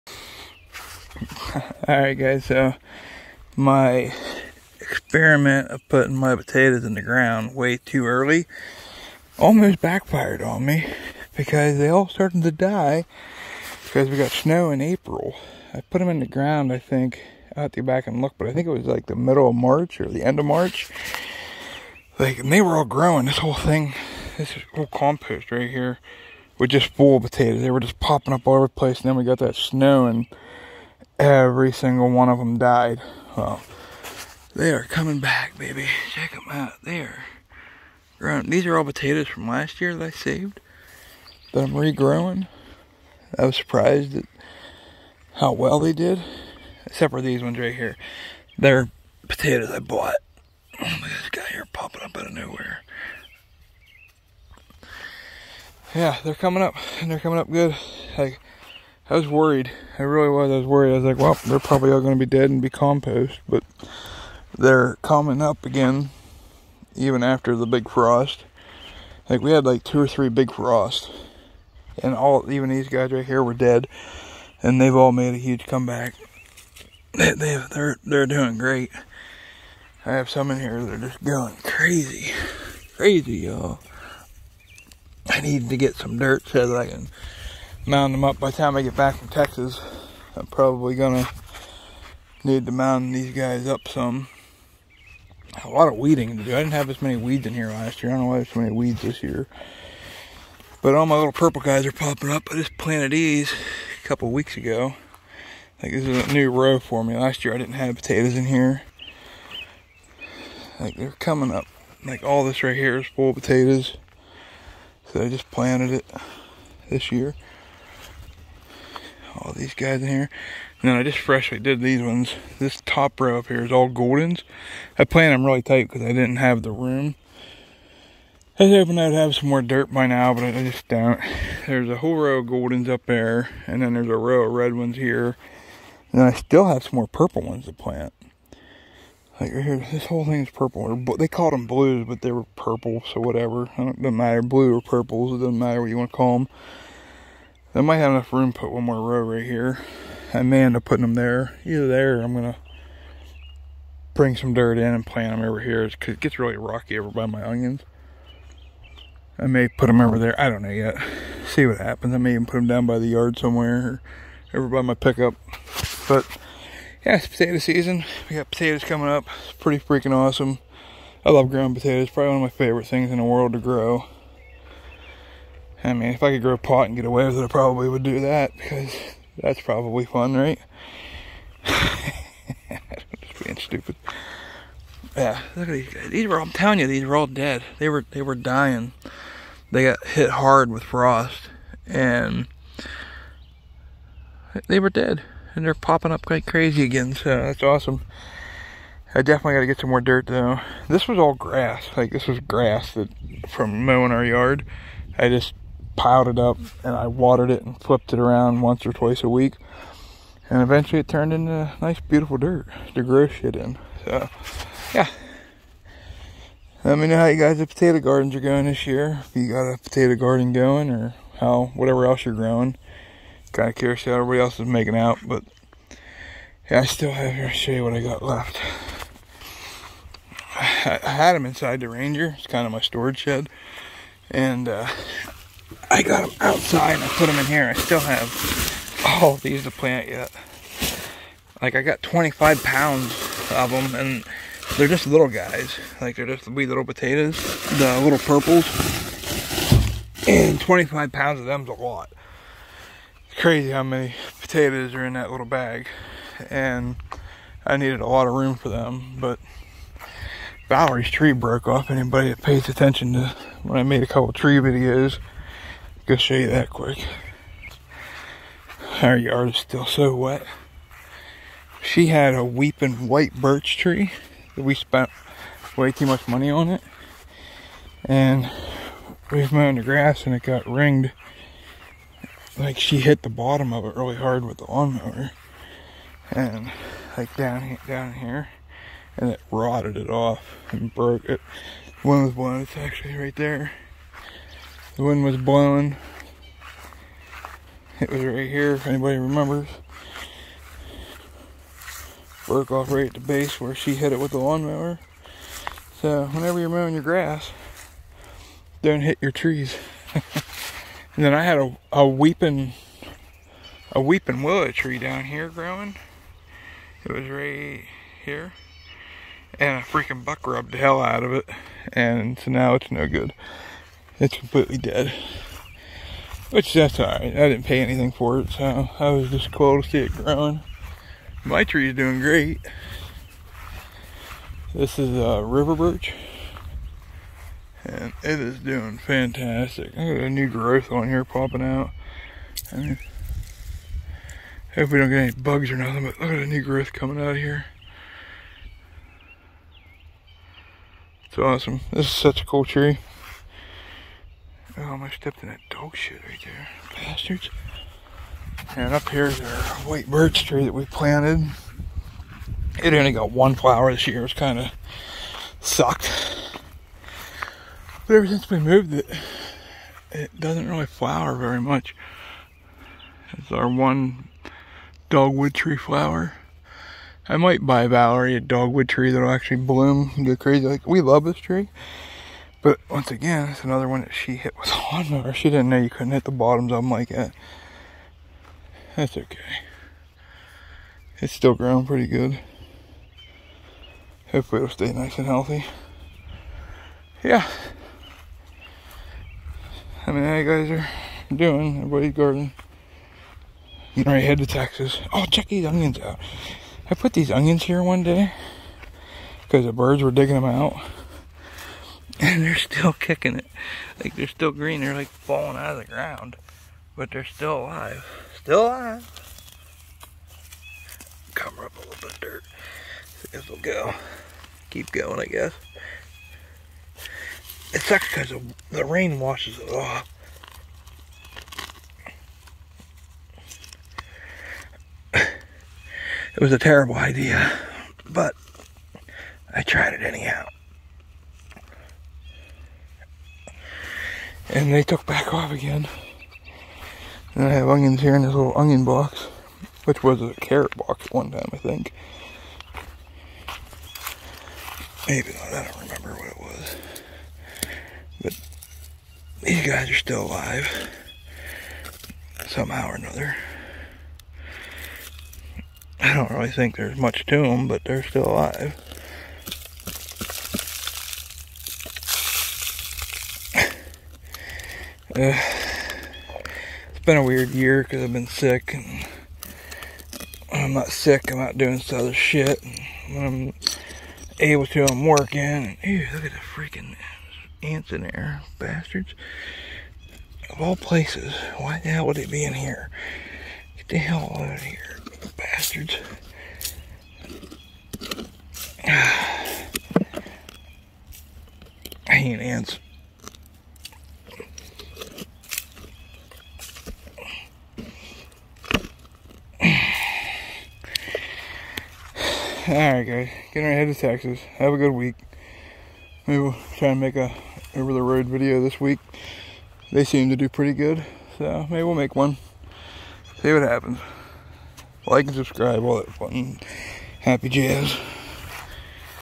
My potato plants all died.., but.. & Weeping birch tree blew over..
so here is my march potatoe video If anyone is interested..